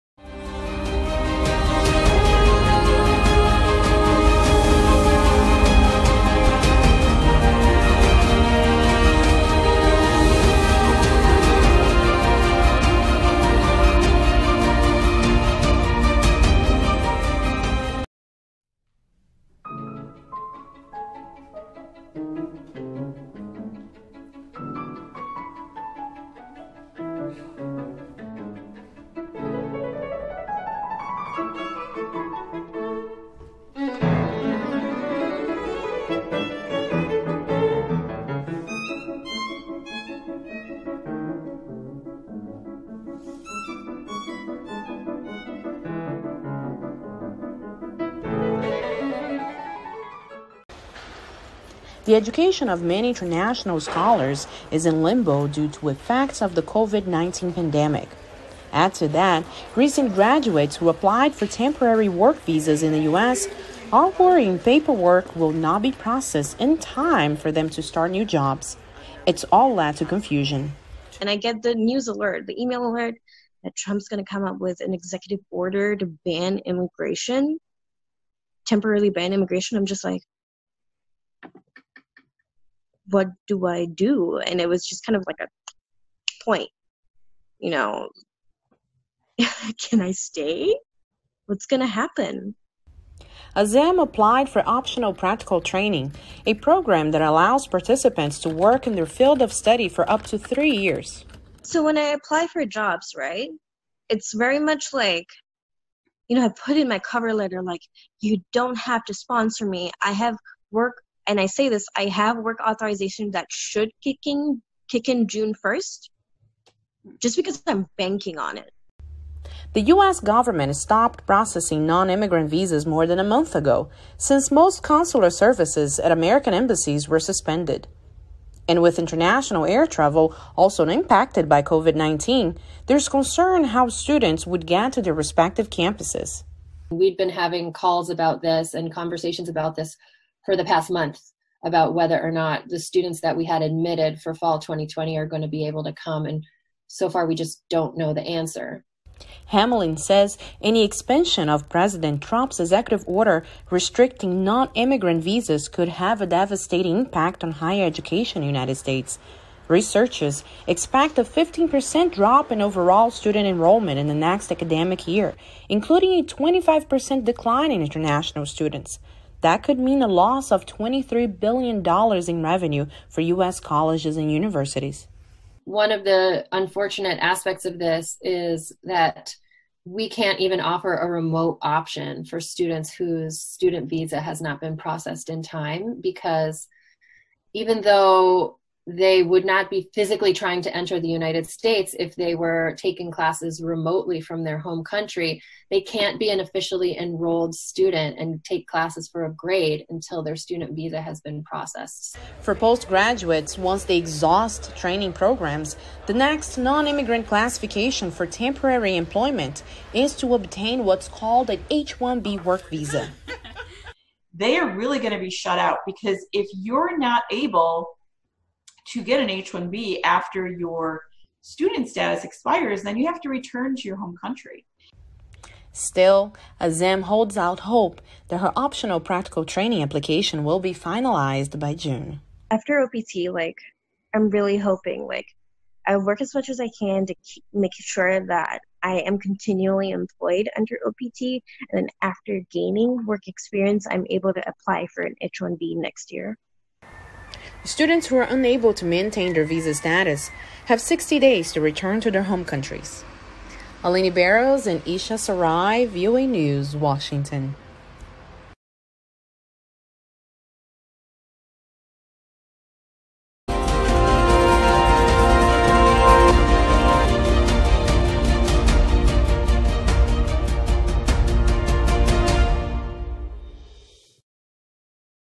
Foreign Students Anxious About Immigration Order
The Trump administration's recent executive order suspending most immigration to the U.S. does not directly impact foreign students. But it has created anxiety and doubts about their ability to stay and work in the United States after graduation. Aline Barros explains~VOA NEWS